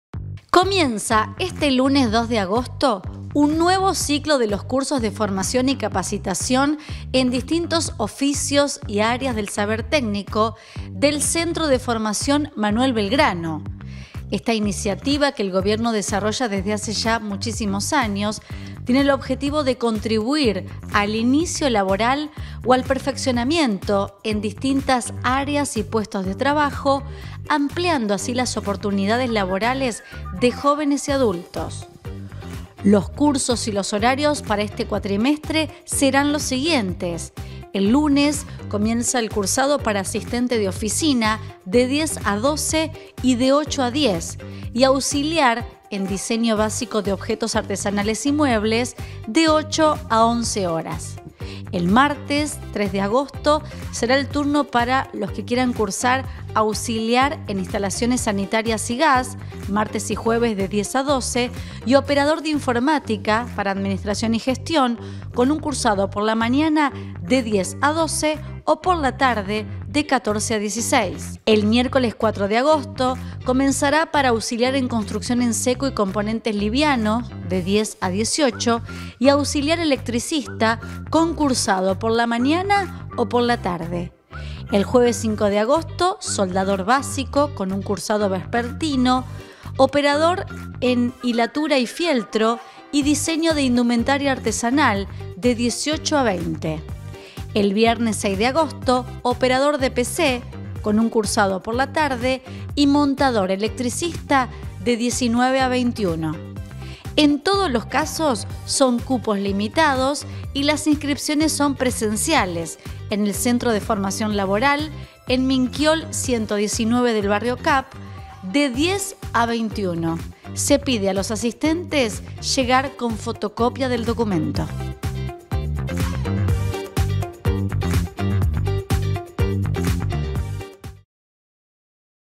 COMIENZAN LOS CURSOS DE FORMACIÓN LABORAL Y OFICIOS
El Centro de Formación Laboral “Manuel Belgrano”, oferta variada desde la carrera de soldador a diseño de indumentaria.